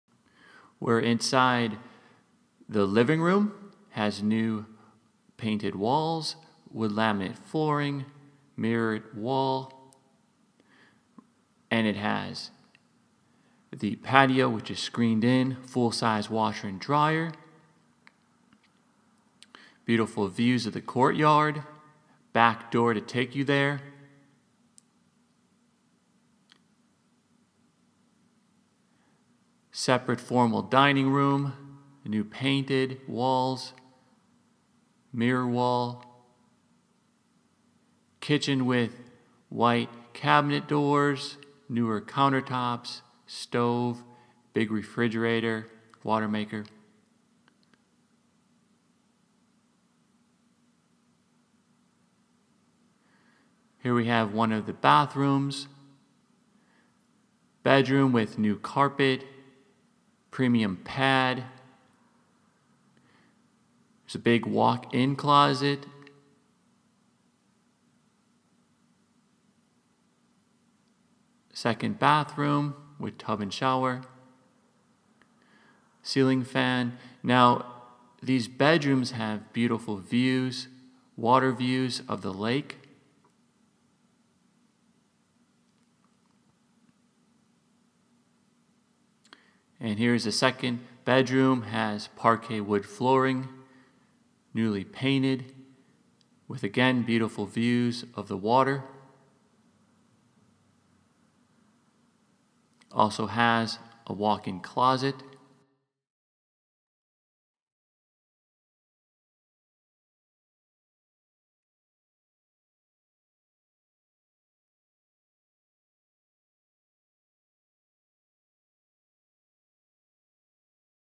Tampa Condo for Rent - 4018 Cortez Dr
This is 2 bedroom 2 bathroom top floor condo for rent, 1138 Sq.Ft. includes screened patio with full size washer and dryer, view of courtyard, separate dining room, kitchen with white cabinet doors, newer counter tops, glass top stove / oven, large refrigerator, pantry food closet, dishwasher, garbage disposal. Large Living room with wood laminate flooring. Master bedroom with new carpet and premium upgraded pad, walk-in closet, bedroom water views of lake, Master bathroom with tub and shower, second bathroom with tub and shower, second bedroom with parkay wood floors, walk-in closet.

Community amenities: basket ball court, tennis court, play ground area, swimming pool.